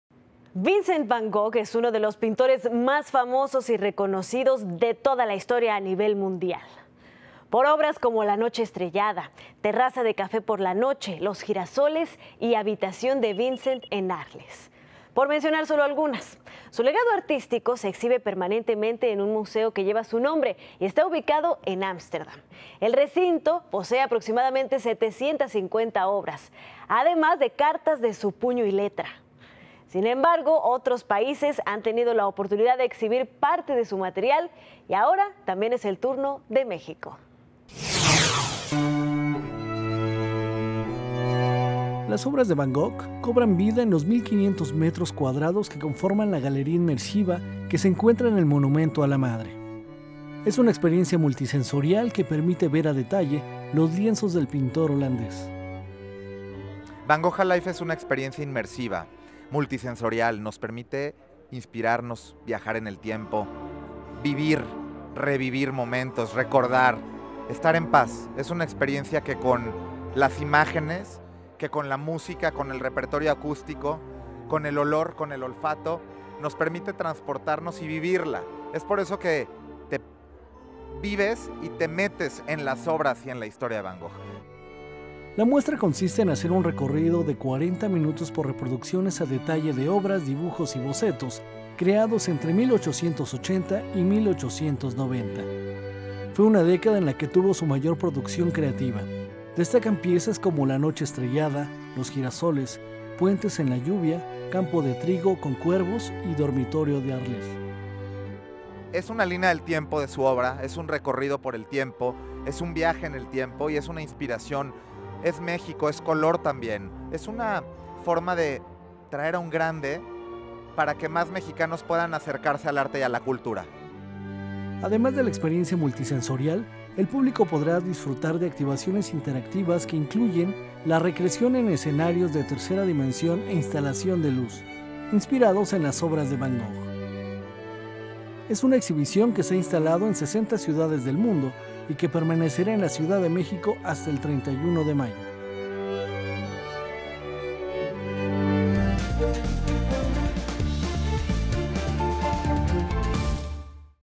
Todo el co­lor de Van Gogh lle­ga a Mé­xi­co con esta mues­tra mul­ti­sen­so­rial
Las obras de Vin­cent van Gogh han co­bra­do vida en los dos mil 500 me­tros cua­dra­dos que con­for­man una ga­le­ría in­mer­si­va en la Ciu­dad de Mé­xi­co. Está ex­pe­rien­cia mul­ti­sen­so­rial per­mi­te ver a de­ta­lle los lien­zos del pin­tor ho­lan­dés.